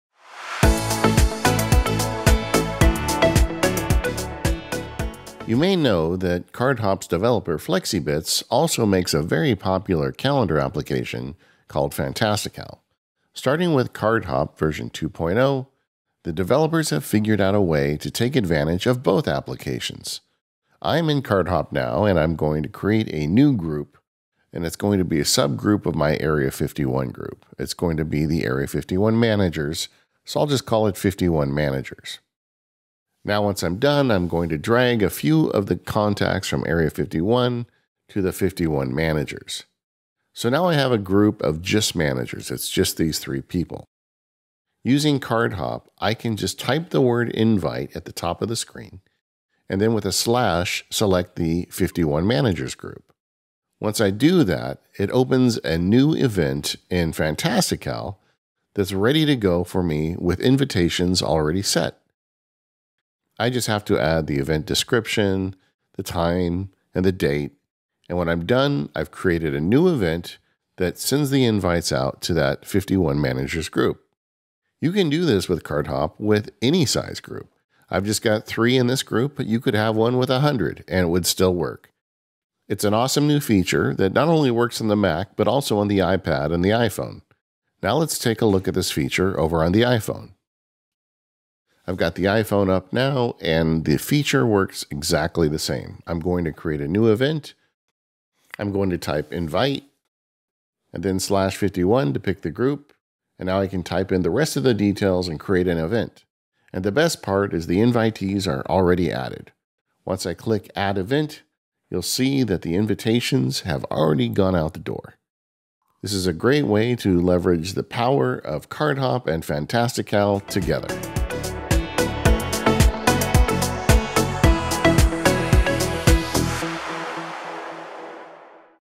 Cardhop - Invites with Fantastical
See how you can use Cardhop to quickly invite people to an event in Fantastical.